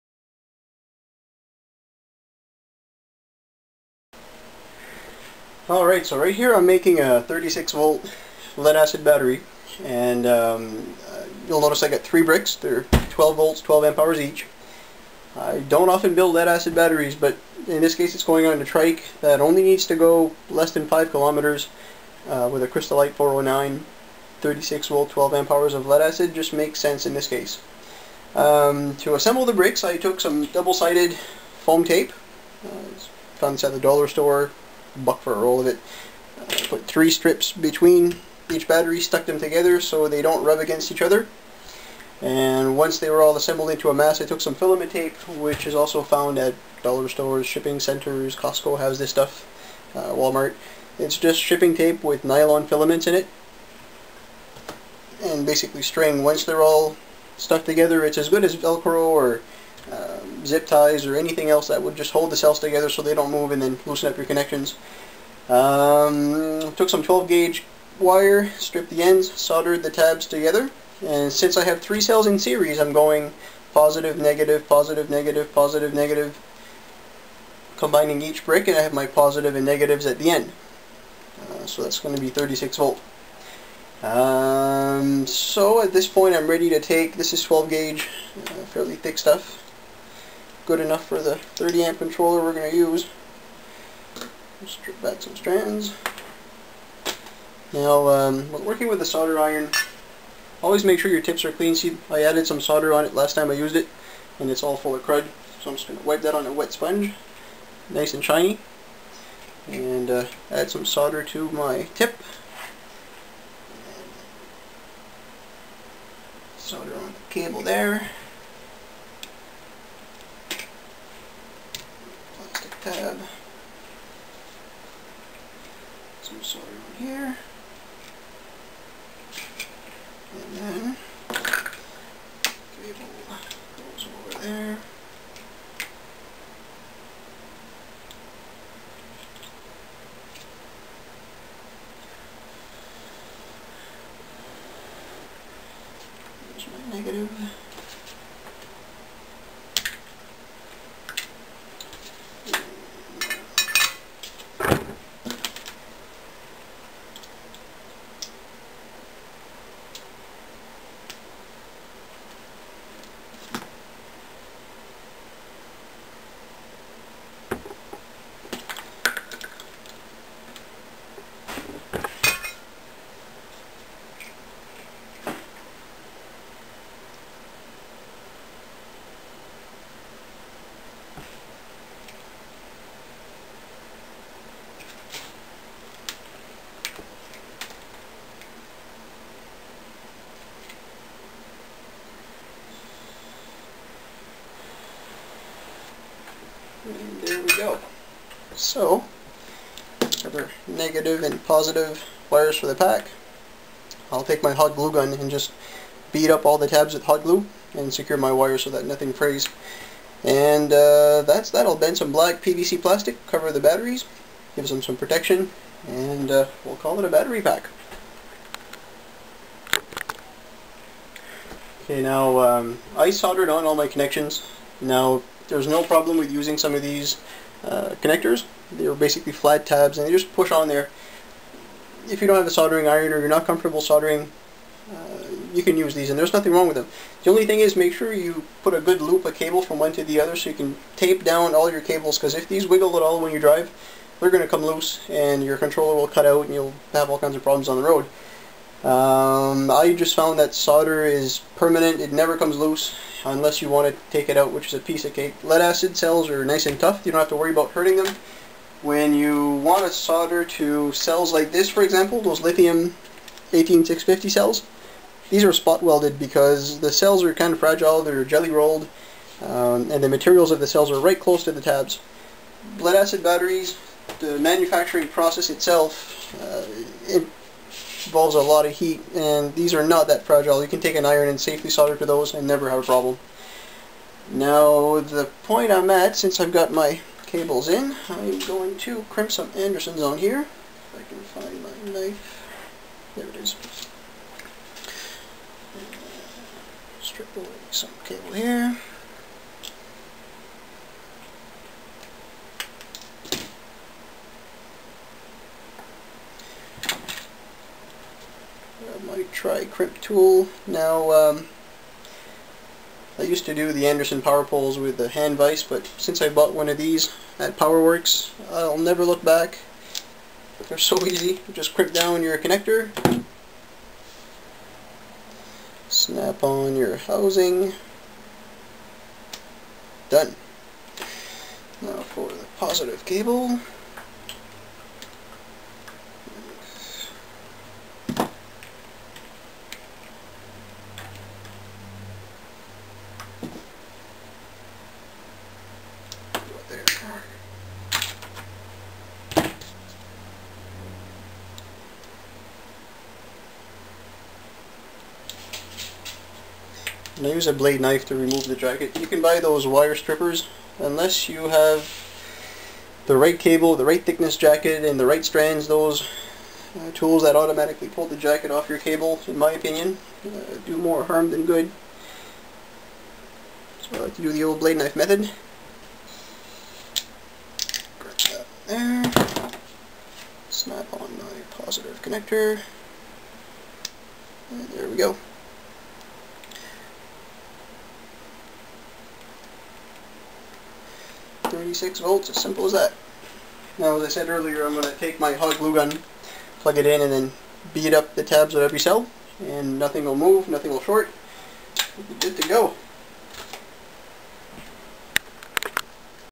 36v SLA 12ah Battery Pack Build
How i put together a 36v SLA battery pack, always remember to remove all jewelry and keep a clean non-conductive area when working with batteries.